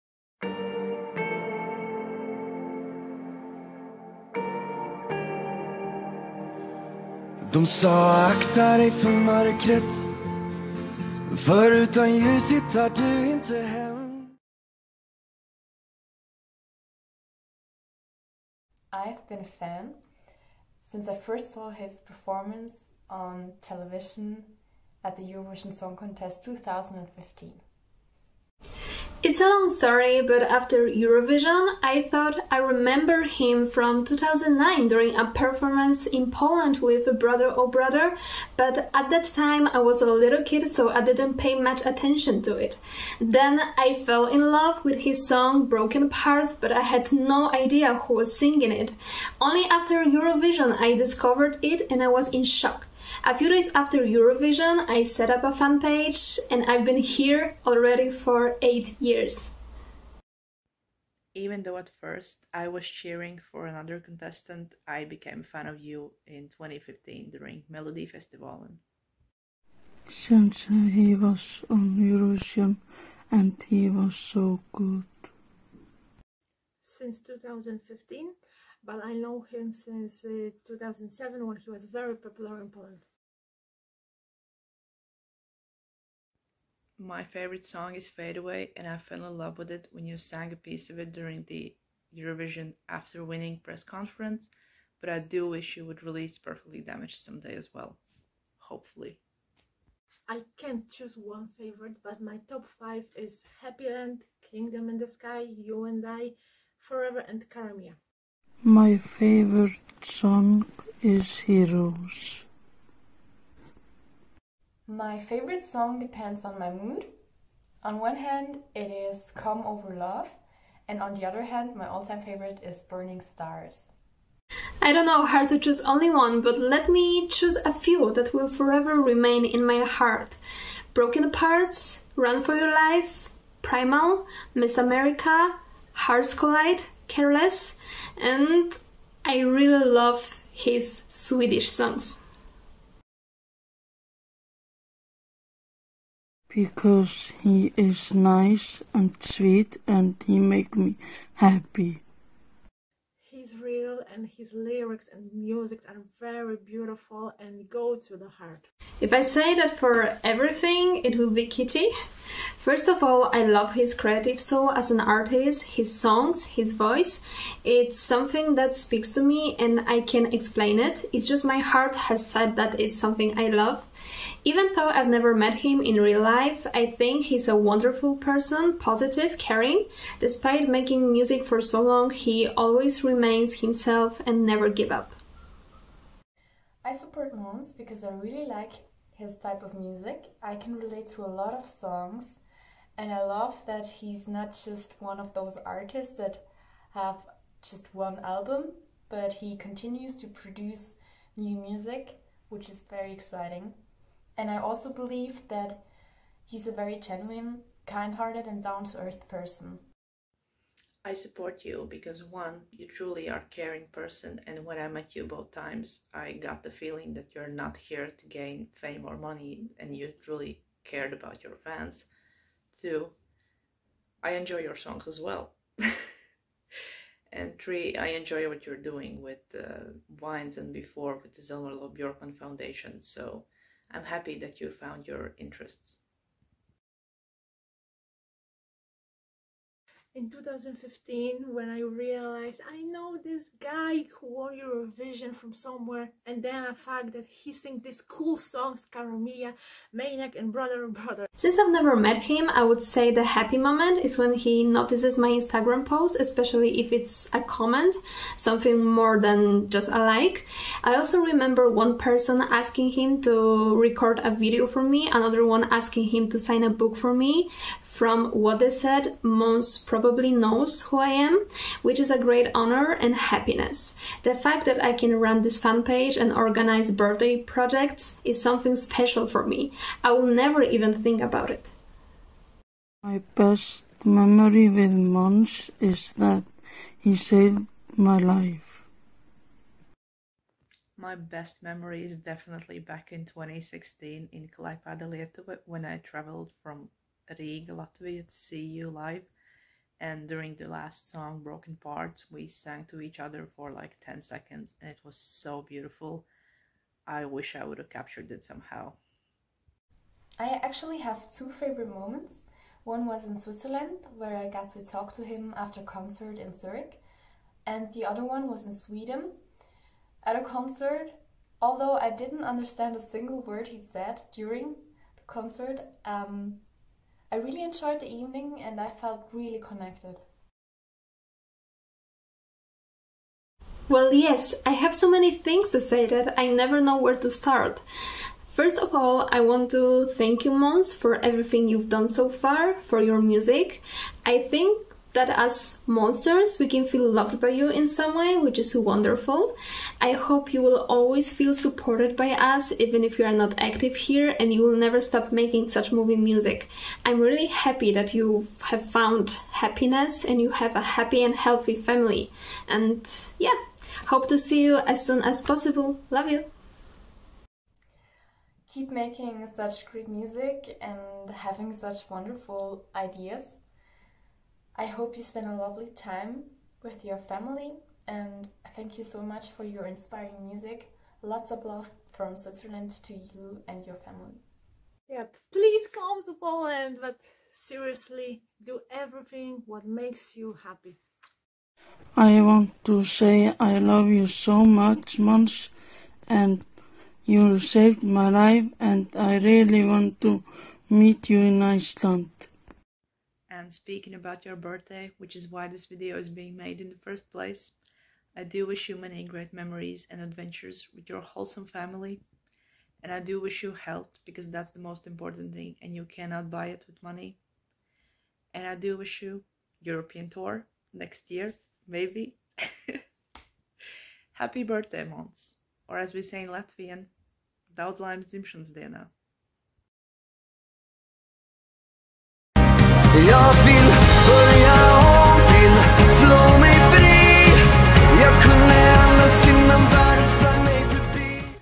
"Interview with Månsters 🎙️" - Birthday surprise for Måns
Birthday surprise from Månsters to Måns called "Interview with Månsters 🎙️" on the occasion of his 37th birthday  💙🎂

The interview was conducted with five Månsters from four countries - Poland, Switzerland, Latvia and Iceland. We shared our own stories as well as our favorite things about Måns. The purpose of this project was to show support and express our gratitude for the past few years!